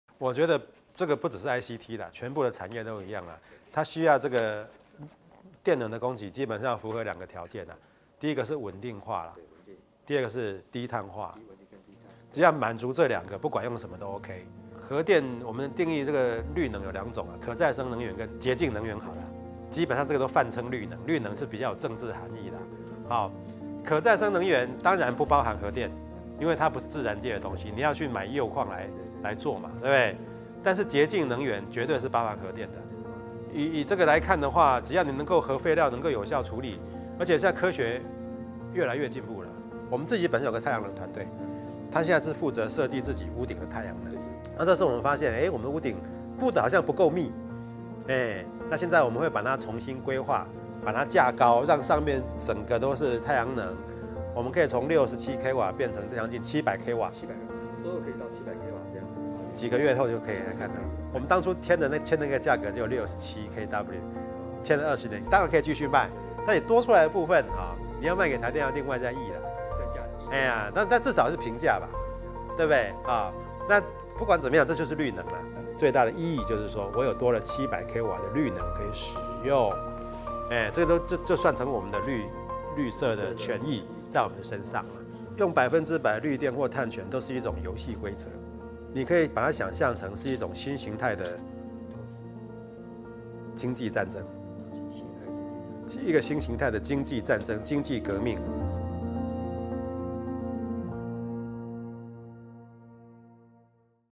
鴻海盼台能源供穩且低碳 不反對核電｜中央社影音新聞
談到台灣能源政策規劃，鴻海集團環保長洪榮聰首次表達看法，認為不僅是鴻海集團，對於台灣任何產業廠商來說，能源穩定供應和低碳化是兩大要件。他進一步表示，若能達到這兩項要件，任何能源發電形式均可考慮；若核廢料能有效處理，不反對採用核能發電。